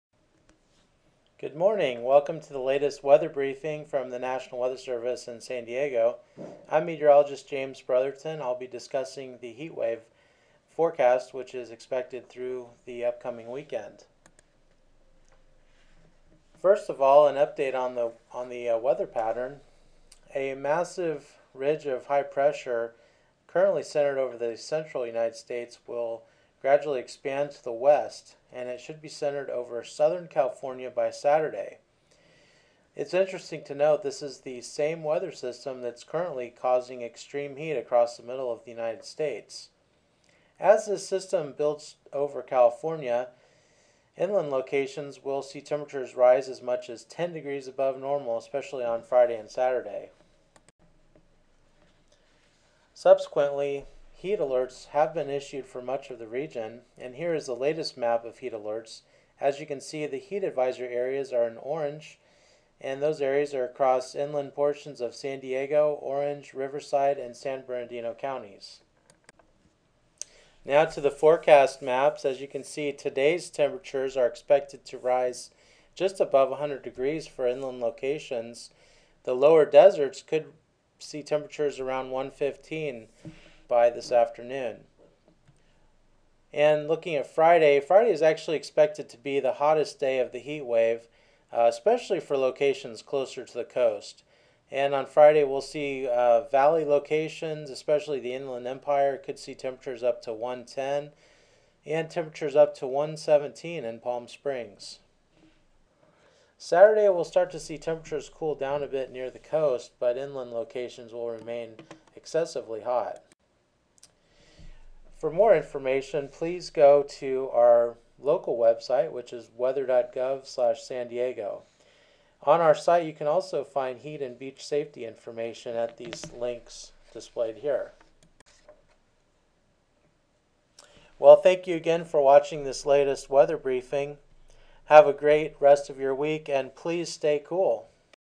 SoCal Heat Wave through this Weekend, July 21-24
This is our latest weather briefing, discussing the developing heat wave event across much of Southern California.